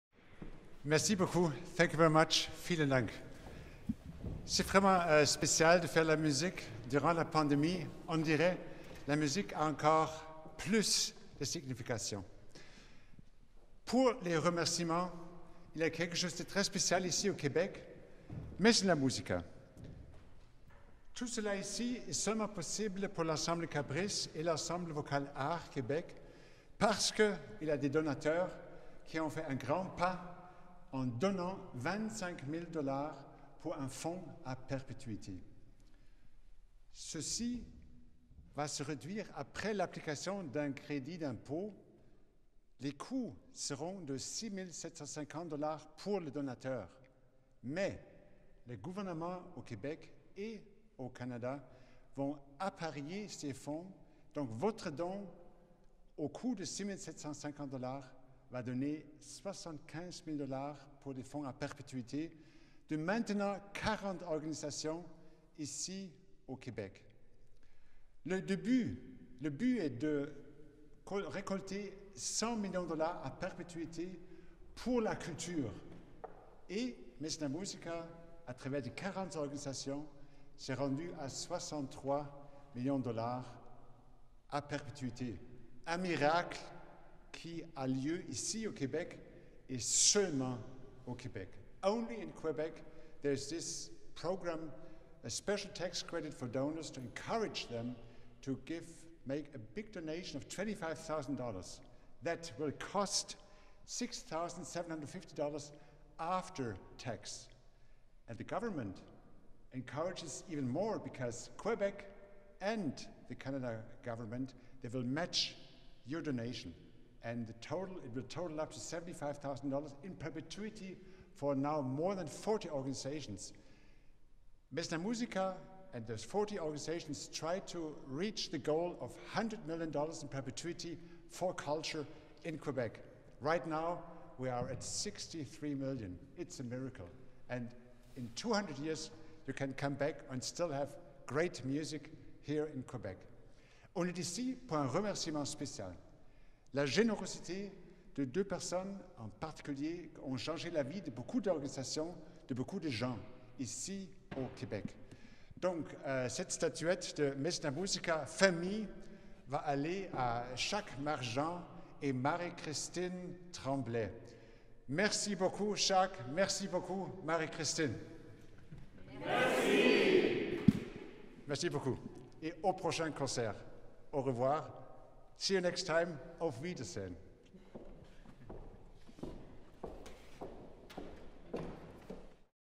Mécénat Musica à perpétuité / in perpetuity: 25 000 $ (Mécénat Musica 142.1)
Mécénat Musica 142.1 - Mécénat Musica à perpétuité: 25 000 $

Mécénat Musica est un programme culturel qui a été créé par et pour des mécènes. Il encourage des individus à faire un don à perpétuité de 25 000 $ à un organisme culturel qui leur tient à cœur et incite des familles de mécènes à faire un don de 250 000 $ ou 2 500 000 $ en appariant ces dons individuels.

Le coût net pour un individu d’un don en argent de 25 000 $ est de 6 750 $* (ou 5 425 $*) après crédits d’impôt grâce, entre autres, au crédit d’impôt additionnel du Québec pour un don important en culture, crédit qui peut être utilisé une seule fois dans la vie.

Mécénat Musica 142.1 - Mécénat Musica in perpetuity: 25 000 $

Mécénat Musica is a cultural program created by donors for donors. It encourages individuals to make an in-perpetuity donation of $25,000 to a cultural organization that is important to them, and it encourages philanthropic families to match these individuals’ donations with donations of $250,000 or $2,500,000.

The net cost to an individual of a $25,000 cash donation after personal tax credits is $6,750* (or $5,425*) and is due, among other credits, to an additional tax credit from Revenue Quebec for one large cultural donation, which can only be claimed by the donor once in his or her lifetime.